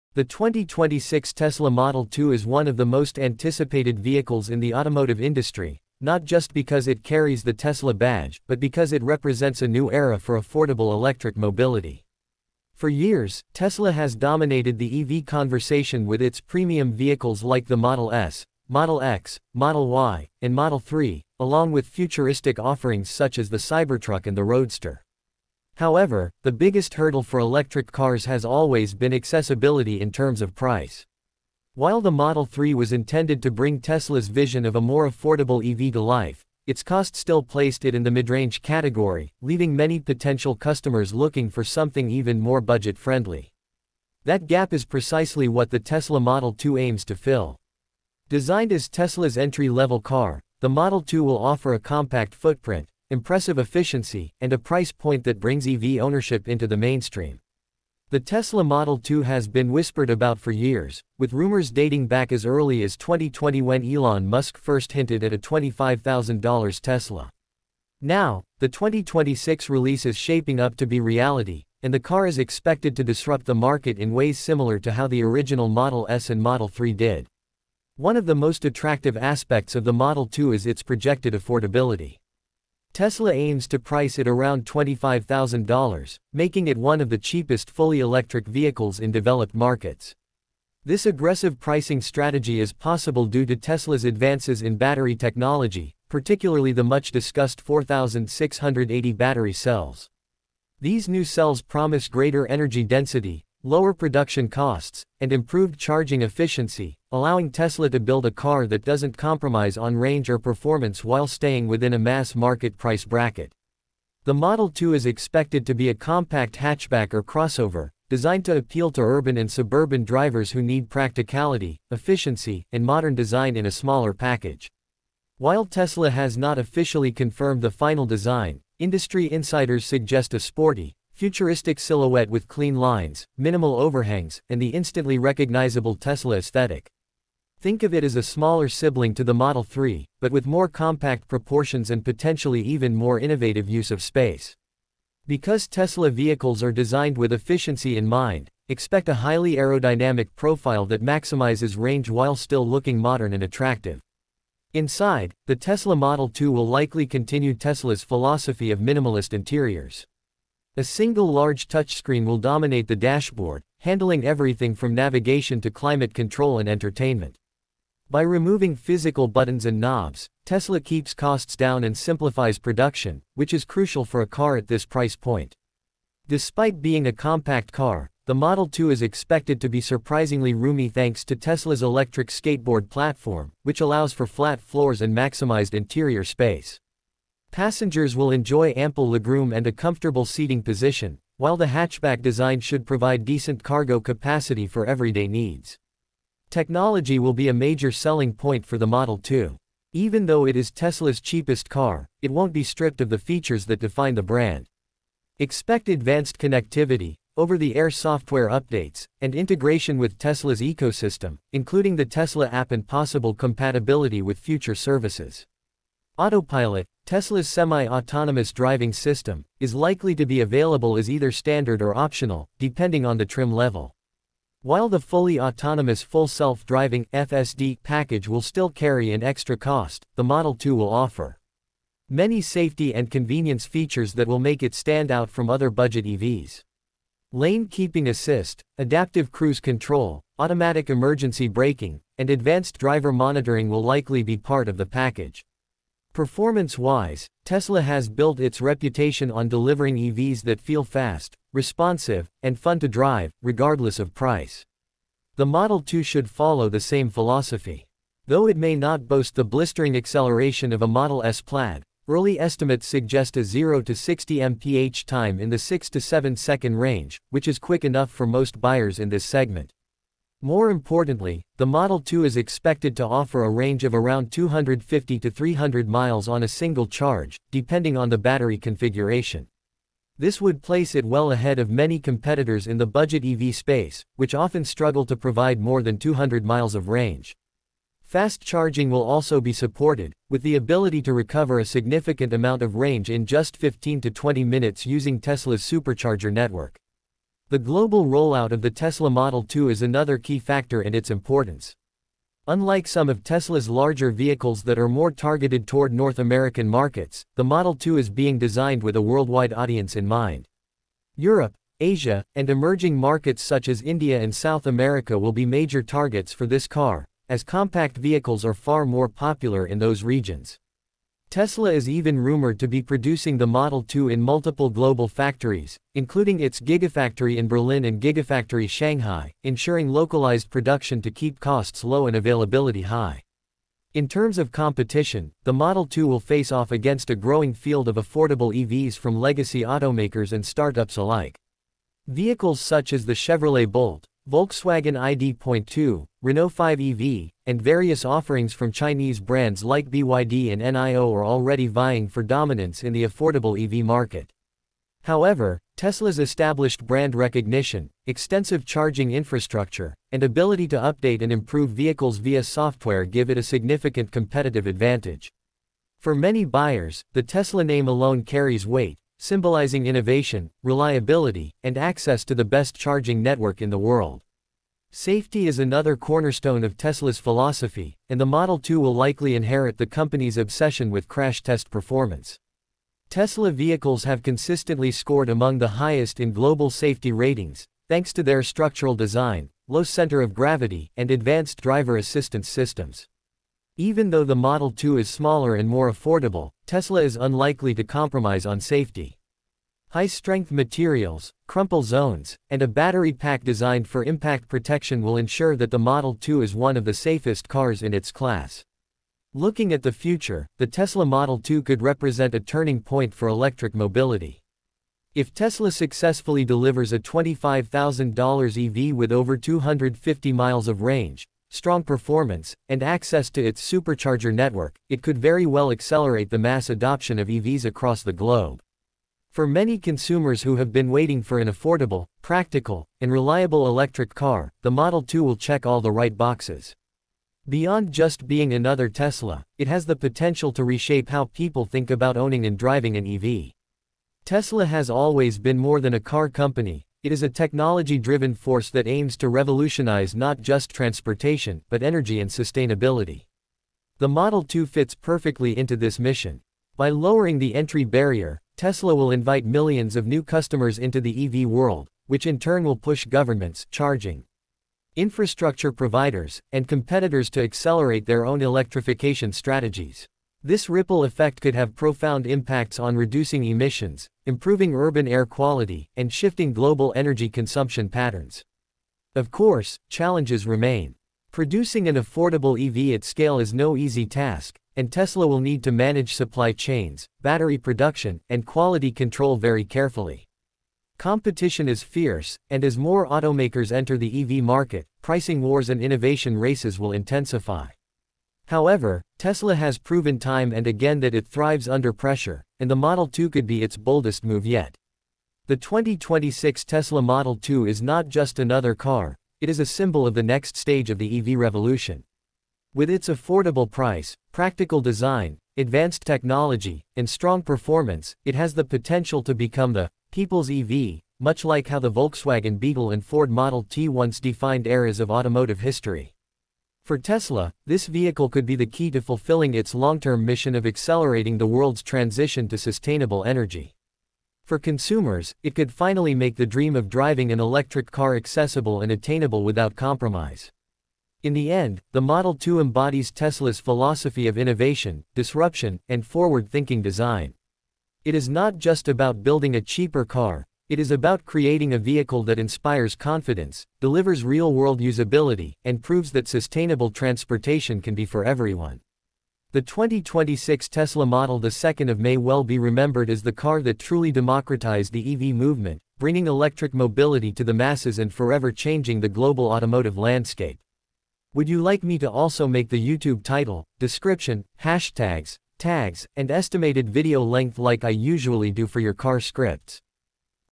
2026 Tesla Model 2 – The $25,000 EV That Changes Everything
2026 Tesla Model 2 – The $25,000 EV That Changes Everything

Description:
The all-new 2026 Tesla Model 2 is here to revolutionize the electric vehicle market. With an affordable price around $25,000, futuristic design, impressive range of up to 300 miles, and Tesla’s latest technology, this compact EV is built to bring sustainable driving to the masses. In this video, we explore the details of the Tesla Model 2 including design, performance, battery technology, Autopilot features, charging capabilities, global production plans, and how it stacks up against its rivals. Is this the true “people’s EV” we’ve all been waiting for?

Timespan (9 min video):

0:00 – Introduction to Tesla Model 2

0:40 – Affordable EV vision and price point

1:40 – Design and styling details

2:30 – Interior, comfort, and technology

3:30 – Battery, range, and charging performance

4:40 – Driving dynamics and performance figures

5:30 – Autopilot and safety features

6:30 – Global markets and production plans

7:20 – Competitors and Tesla’s advantage

8:20 – Why Model 2 could change EV history

9:00 – Conclusion and final thoughts

Do you want me to also prepare a clickbait-style alternative title option for higher engagement, like “2026 Tesla Model 2 – The $25,000 EV That Destroys the Competition”?